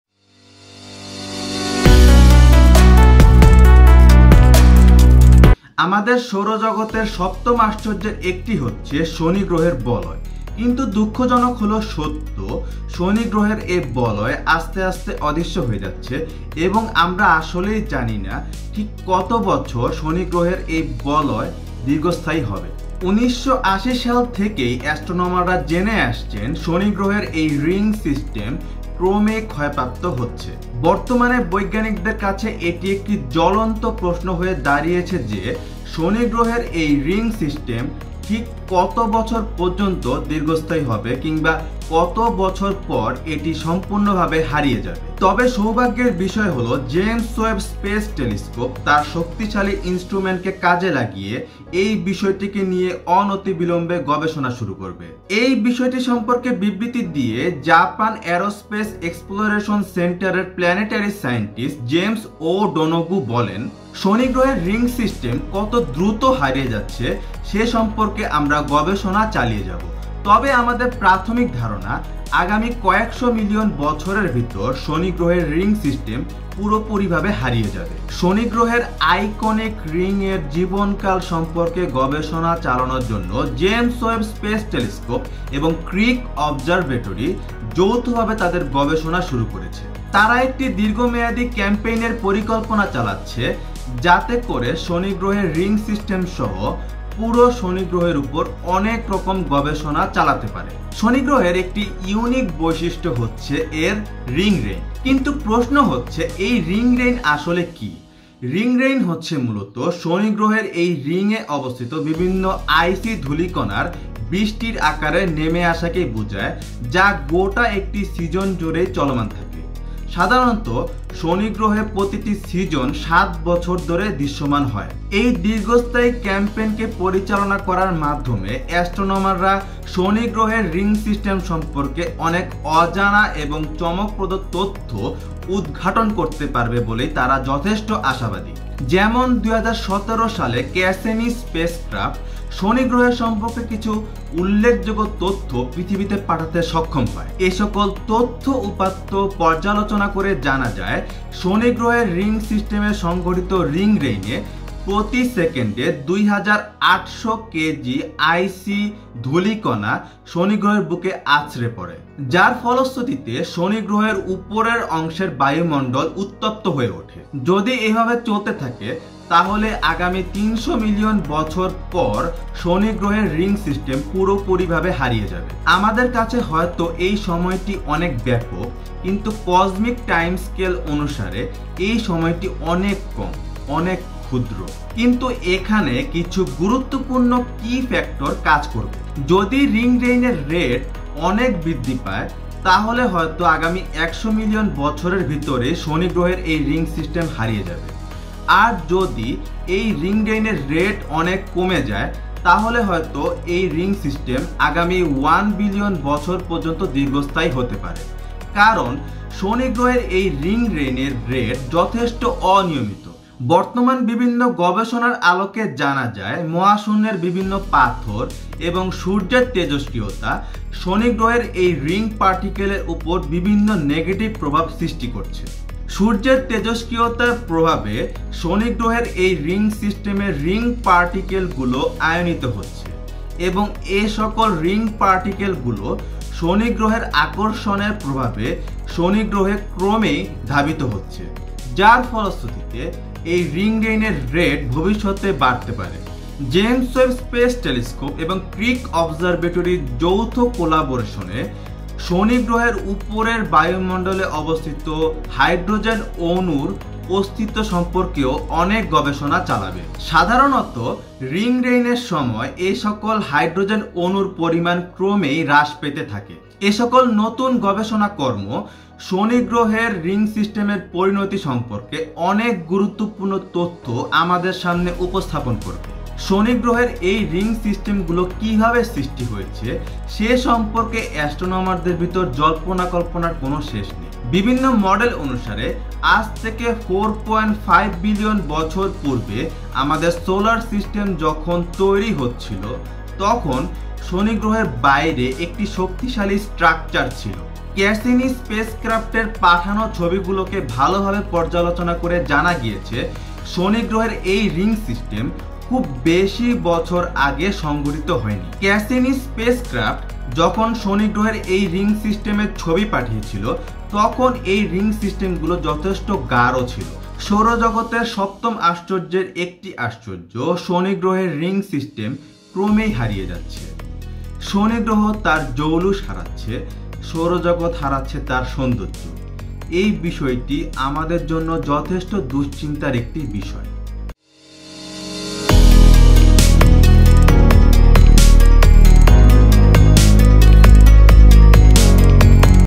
Saturn's Rings are Disappearing! But Why?
Some of the world's most powerful observatories are poised to study the "ring rain" phenomenon. (SPACE.COM)

Do not DOWNLOAD / REUPLOAD or COPY this Video

All the Information were Collected from SPACE.COM, WIKIPEDIA.COM
All the Used Clips were Collected from PIXABAY.COM